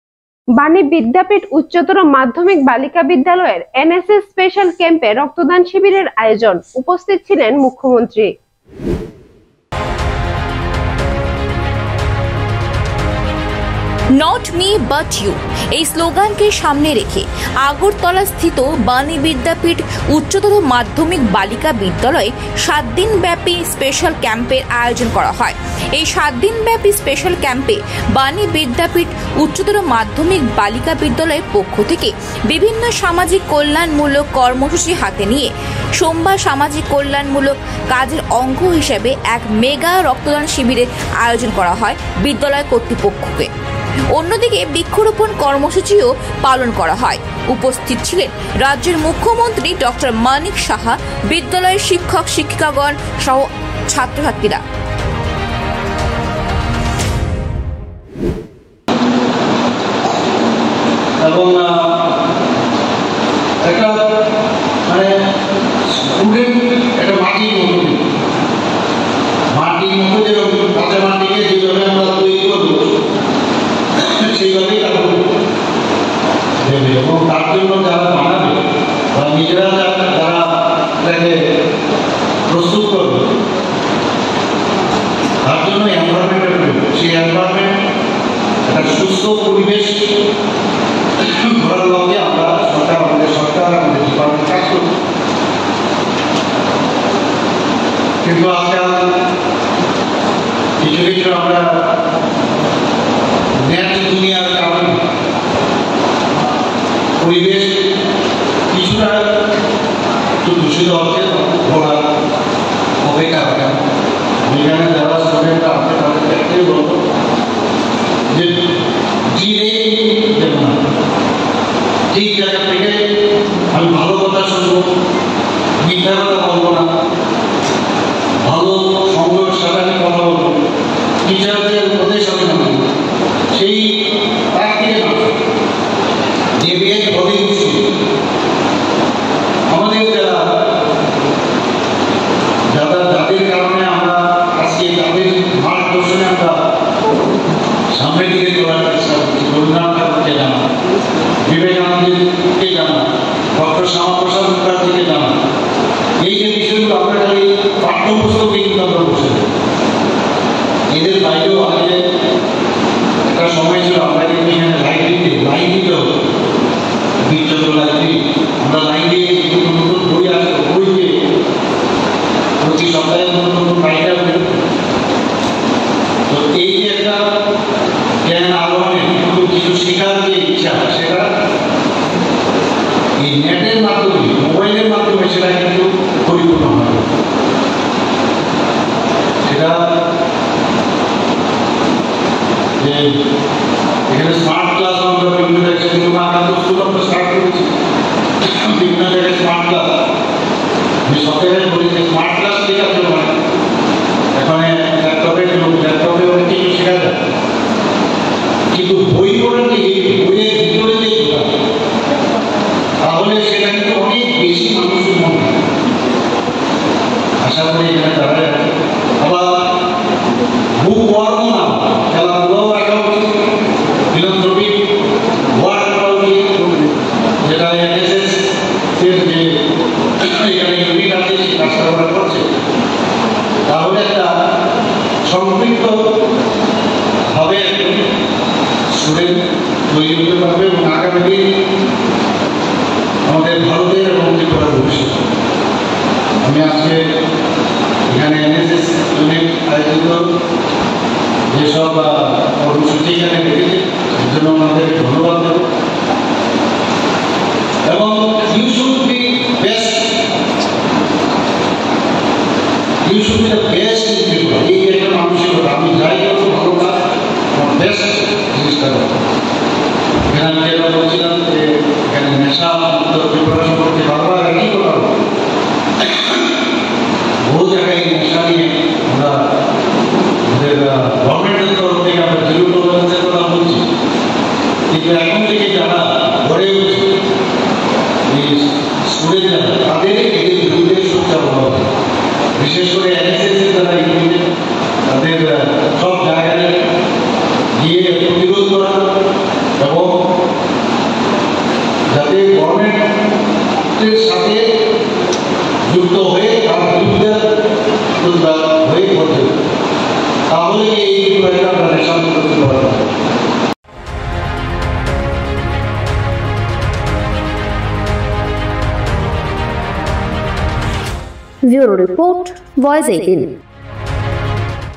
Agartala : NSS স্পেশাল ক্যাম্পে রক্তদান শিবিরের আয়োজন উপস্থিত মুখ্যমন্ত্রী | VOICE 18
VOICE 18
Name of Production House: FLAXEN MEDIA WORKS